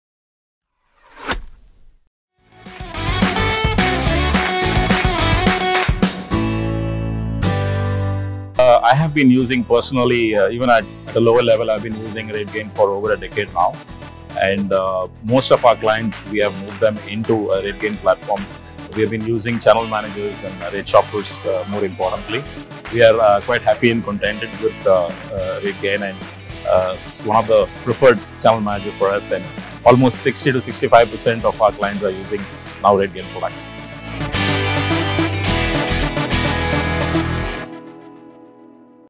RezGain - Customer Speak
Find out why RezGain is the preferred channel management platform for Revopt.